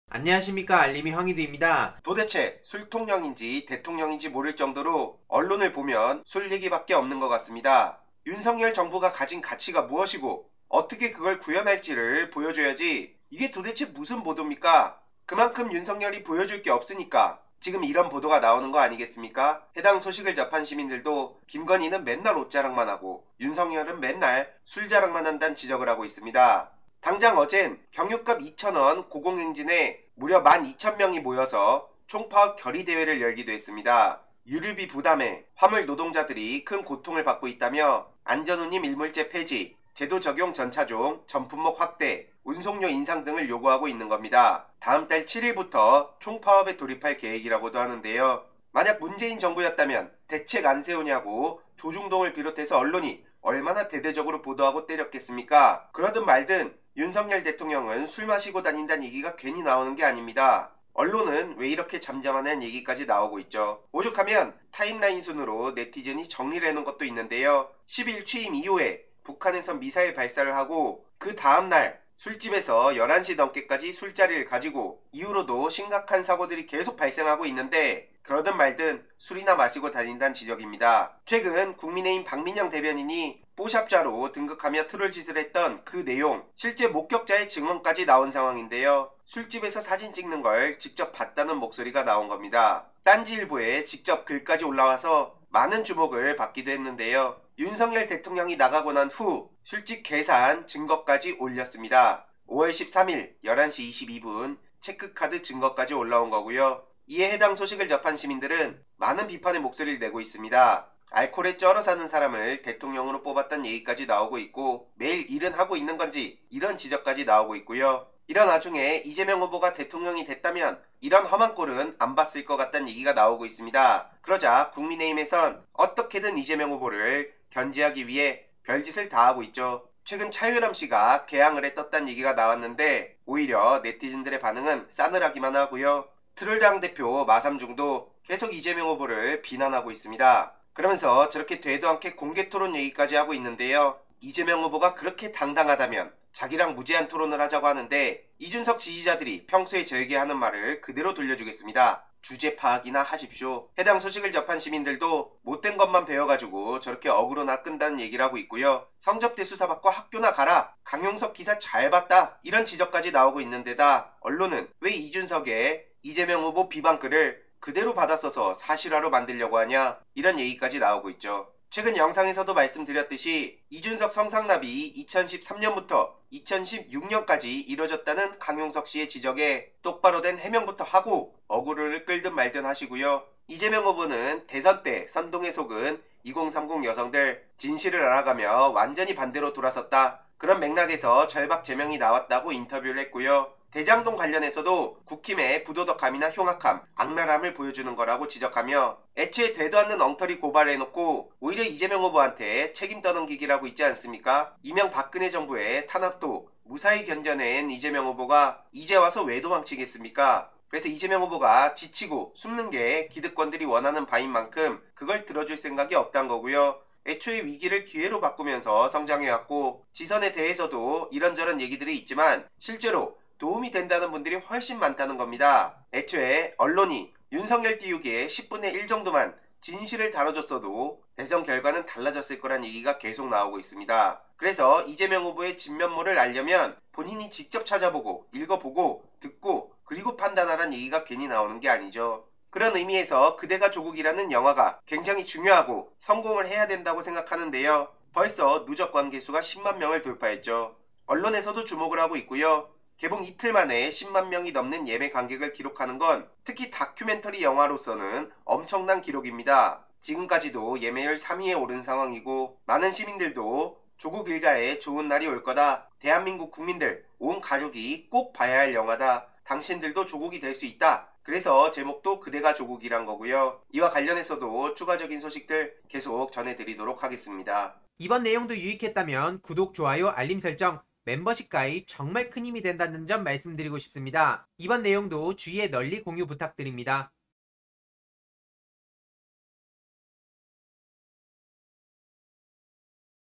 '그대가 조국' 흥행, 벌써 10만 돌파했다! 목격자 등장 "윤석열, 그날 술집에서 사진 찍는 것 직접 봤다" - 이재명 후보 비난하며 어그로끄는 이준석.. 네티즌 "주제파악"
영상 설명 : 그대가 조국 다큐멘터리 예매가 벌써 10만명을 돌파한 상황입니다. 윤석열 대통령 술자리 비판과 목격자 등장, 이재명 후보 비난하며 어그로끄는 이준석 대표 비판 등을 요약해봤습니다.

굿모닝충청 - 네티즌 “윤 대통령, 그날 술집서 사진 찍는 것 내가 직접 봤다”

[굿모닝충청=서울 정문영 기자]  최근 ‘심야 술집 만취’ 의혹을 사고 있는 윤석열 대통령의 음주사실 등을 입증하는 목격자의 증언이 나와 주목된다.

27일 한 인터넷 커뮤니티에는 윤석열 대통령 술 먹고 나갈 때 찍힌 사진에 대해서라는 글이 올라와 설왕설래가 한창이다.

네티즌은 이날 윤 대통령이 지난 13일 밤 11시까지 자택 인근 술집에서 거나하게 술을 마시고 고객들과 함께 사진 찍은 상황을 직접 목격했다며, 관련 사실을 소상하게 털어놓았다. 제보자가 제공한 ‘만취 사진’을 공개한 열린공감TV 보도가 사실임을 입증하는 윤 대통령의 알리바이에 관한 증언인 셈이다.

그는 “서초동 아크로비스타와 삼풍아파트에 사는 사람이면 바로 알 수 있는 술집인데, 술집이름은 버XXX”라며 “5월 13일 밤 10시 조금 넘어서 들어갔을 때 손님 테이블은 2개 정도였고, 제가 앉으려고 했던 테이블은 예약이 되어 있다면서 다른 곳으로 안내한 곳이 입구 근처였다”고 적었다.

이어 “그래서 ‘이 늦은 시간에 누가 예약을 했을까’ 궁금해서 병맥주 먹으면서 지켜봤는데, 10시 15분쯤 검은색 정장 입은 사람이 1명씩 들어오더니 테이블 1개당 1명씩 앉았고, 제가 본 인원만 6명이였다”며 “그리고 조금 지나서 윤 대통령과 같이 한 분이 들어오는 걸 보고 정말 깜짝 놀랐다”고 덧붙였다.

그는 “저는 검정색 정장 입은 한 분에게 가서 (대통령) 싸인 받을 수 있냐고 물어보자, 그가 ‘대통령님이 쉬러 오신 거라서 싸인 받는 건 힘들 거 같다’고 말했다”며 “대통령이 나가실 때 다른 테이블에 있는 손님이 사진 찍고 싶다고 하니 흔쾌히 허락하고 같이 사진을 찍었는데, 아마 그 사진이 유출된 걸로 보인다”고 말했다.

그리고는 “윤 대통령이 나가고 난 뒤 제가 술집에서 계산했을 때 시간을 증거로 올린다”며 결제정보가 찍힌 자신의 체크카드 사용내역을 올렸다.

결제내역에는 '05.13, 23:22'로 정확하게 표기돼 있어, 네티즌의 주장이 사실이라면 윤 대통령의 ‘심야 만취사실’은 부인하기 어려운 팩트로 보인다. 이날은 윤 대통령이 대한민국 20대 대통령으로 취임한 지 보름 남짓, 북한의 미사일 발사 후 딱 하루가 지난 시점이다.

이 글을 본 네티즌들의 설왕설래가 분주한 가운데 그 중 몇 개만 인용하면 다음과 같다. 비판일색이다.

문화일보 - '그대가 조국' 예매 10만 명 돌파..서울대 사직 여부 논란은 계속

“서울대와 일부 언론, 저를 거짓말쟁이 만들어

월급 계속 받자고 학교에 남아 있는 것 아냐” 반박

조국 전 법무부 장관의 취임부터 검찰 수사, 부인 정경심 전 동양대 교수의 재판 과정 등을 조명한 다큐멘터리 영화 ‘그대가 조국’이 개봉 이틀 만에 10만 명 넘는 예매관객을 기록하며 돌풍을 일으키고 있다. 예매율도 두 자릿수를 기록하면서 다큐멘터리 영화의 성공 사례를 만들지 이목이 집중되고 있다. 자신을 다룬 영화에 대한 대중들의 관심이 높지만, 조 전 장관은 자신의 서울대 사직 여부를 놓고 학교 측과 공방을 벌이는 등 심기가 불편한 모양새다.

26일 영화진흥위원회의 영화입장권 통합전산망(KOBIS)에 따르면, ‘그대가 조국’은 이날 오전 4시 기준 17.3%의 예매율을 기록하며 예매율 순위 3위에 올랐고 10만4401명이 예매했다. 높은 예매율 덕분에 매출액도 8억9030만 원을 찍었다. SNS에도 “많이들 보러 가시라” “영화를 보며 이상할 정도로 자꾸만, 예수의 고난과 부활 행적을 오버랩시키게 됐다”는 관람평들이 속속 올라오고 있다.

앞서 이 영화 제작사인 ‘켈빈클레인프로젝트’는 대형 멀티플렉스 상영관을 확보하기 위해 지난달 25일부터 크라우드펀딩 플랫폼 텀블벅에서 모금활동을 펼쳤다. 그 결과 애초 목표치였던 5000만 원의 52배에 해당하는 26억1091만1000원을 모았다. 후원에는 5만1794명이 참여했다. 제작사 측은 CGV, 롯데시네마, 메가박스 등 대형 멀티플렉스 상영관을 다수 확보한 데 이어, 서울 광화문 사거리 전광판에 영화 개봉을 알리는 옥외광고를 게시했다. 앞으로 상영관 숫자도 더 늘어날 전망이다.

‘그대가 조국’은 조 전 장관이 법무부 장관으로 지명된 2019년 8월 9일부터 장관직을 사퇴한 10월 14일까지의 일을 다루며 이후 진행된 검찰수사, 정 전 교수에 대한 재판 과정도 담았다. 조 전 장관을 지지하는 목소리를 내온 이들이 대거 출연해 검찰 수사와 법원 판결, 언론 보도 행태를 비판한다. 연출은 영화 ‘달팽이의 별’로 아시아 최초이자 한국 최초로 암스테르담 국제 다큐멘터리 영화제 장편 부문 대상을 받았던 이승준 감독이 맡았다.

한편, 조 전 장관은 현재 자신의 교수직 사직 여부를 둘러싸고 서울대와 진실공방을 벌이는 중이다. 앞선 25일 황보승희 국민의힘 의원실은 조 전 장관의 사의 표명 여부에 대해 질의해 “조국 교수는 서울대에 사직 의사를 표명하지 않았음을 알려드린다”는 답변서를 서울대 교무과에서 보내왔다고 밝혔고, 이 사실이 언론에도 보도됐다.

조 전 장관은 이날 자신의 페이스북에 “저는 서울대 법학전문대학원 인사 최고책임자에게 분명하게 ‘사직’ 의사 표명을 하며 ‘사직서’를 제출하겠다고 말했다”며 “이 책임자는 이를 서울대 본부의 최고위 관계자에게 보고했다고 저에게 알려줬는데 최고위 관계자는 ‘사직’ 수용 여부에 대하여 공식적 답을 주지 않았다”고 밝혔다. 그는 “그래서 지금까지 ‘직위해제’ 상태가 유지되고 있는 것인데 서울대 본부 및 일부 언론은 ‘사직서가 제출되지 않았다’는 점만 부각시키면서 저를 거짓말쟁이로 만들고 있다”며 “계속 저를 거짓말쟁이로 몰면 두 관계자의 실명을 공개하겠다. 저는 기존 월급의 몇 프로를 계속 받기 위해 학교에 남아 있는 것이 아니다”라고 날 선 반응을 보였다.

노기섭 기자

이와 관련한 내용들을 핵심만 간략하게 요약해봤습니다.